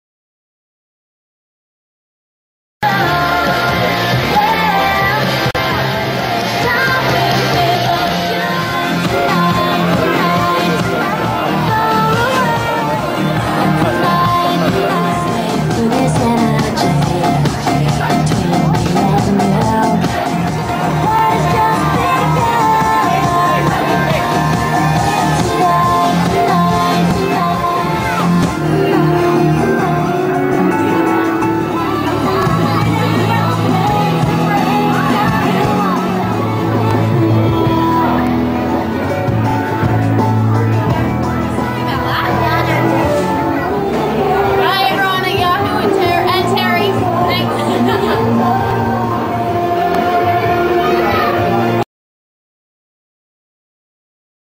H2O Just Add Water - Yahoo Group Instore
Target at Penrith Plaza saw h20 fever,as the Mermaids alter ego`s Phoebe Tonkin,Claie Holt & Cariba Heine enchanted the Fans.